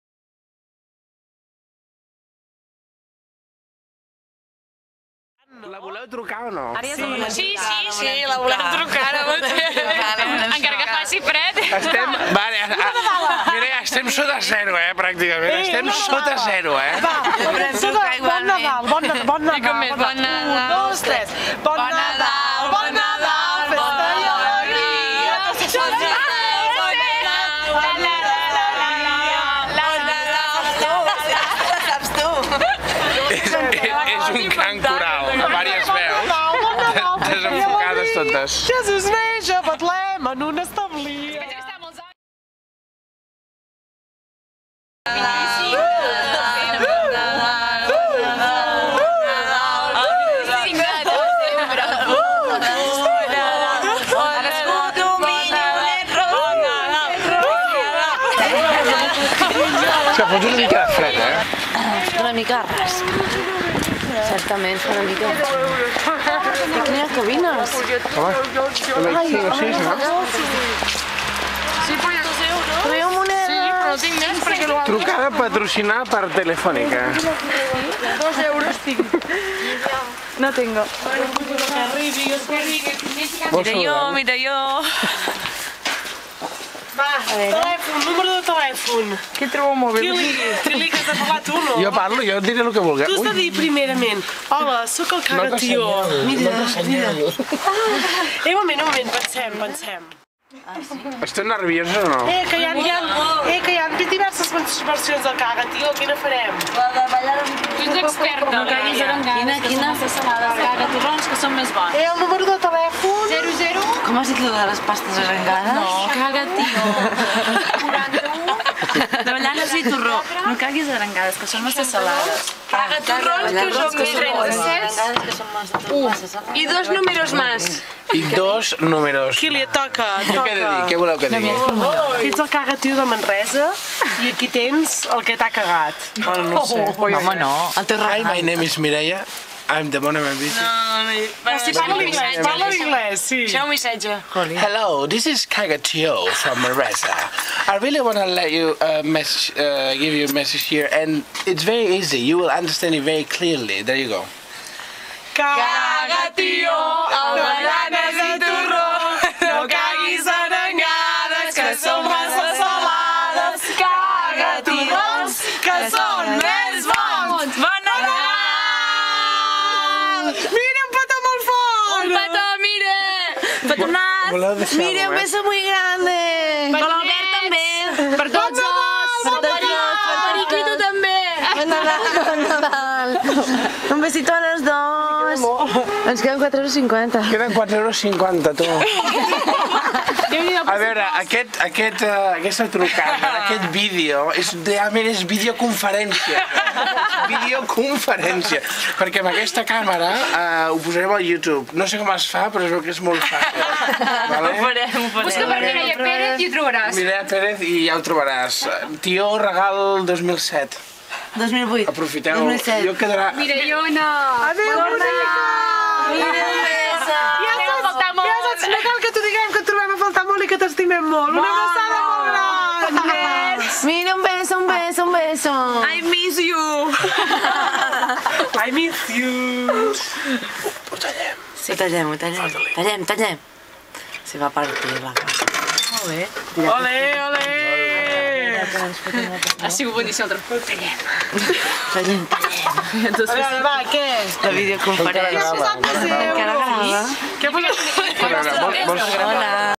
Trucada per la Mireia desde Manresa
Amigues trucant a la Mire a Australia Nadal 07...